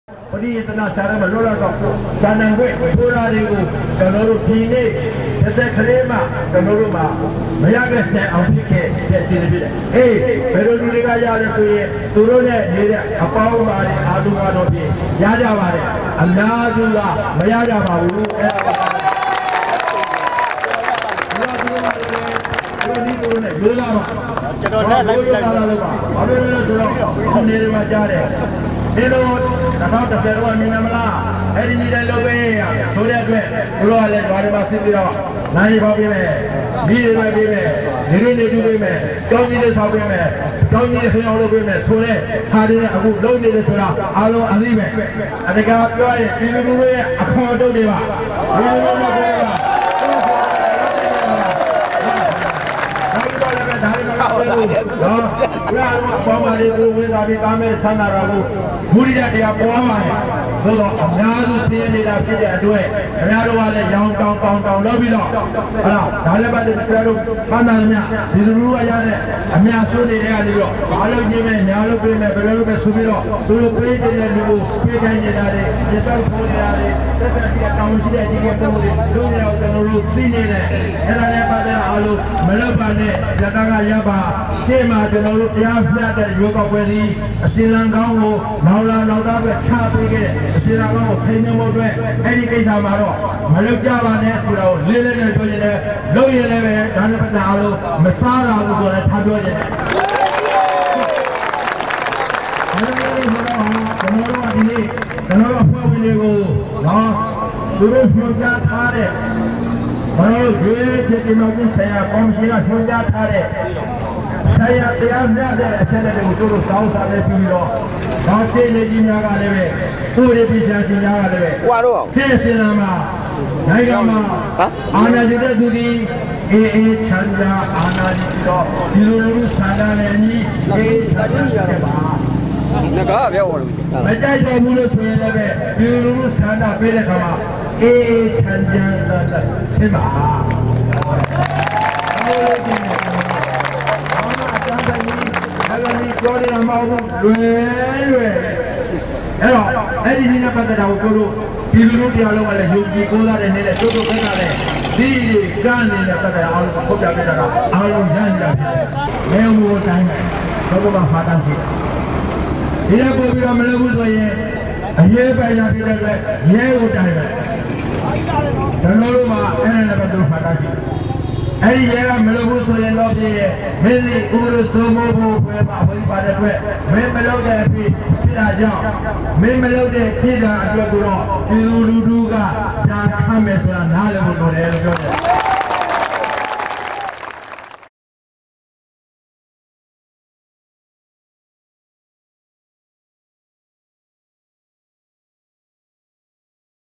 20120216 U Tin Oo Mingalardone
အမ်ိဳးသားဒီမုိကေရစီအဖြဲ႔ခ်ဳပ္ နာယက ဦးတင္ဦးရဲ႕ မဂၤလာဒုံၿမိဳ႕နယ္ မိန္႔ခြန္း